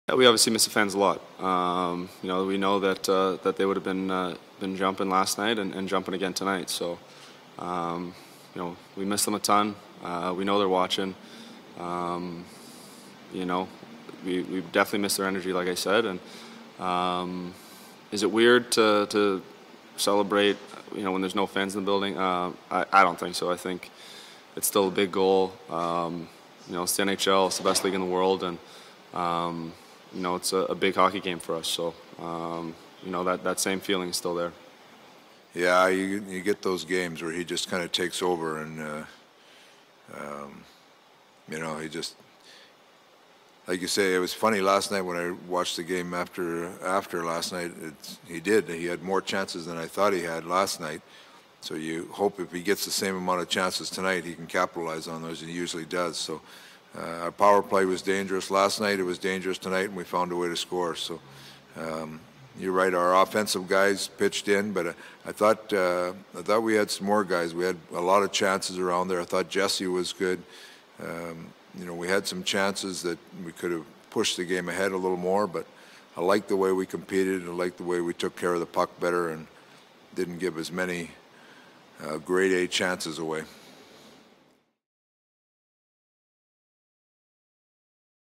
McDavid scores three times to lead Oilers to win over Canucks
McDavid was indefensible with his seventh regular-season hat-trick on nine shots and also had an assist on Nugent-Hopkins' first of two, while teammate Leon Draisaitl, not to be outdone, added four helpers as the Oilers avenged their opening night loss with a 5-2 win Thursday.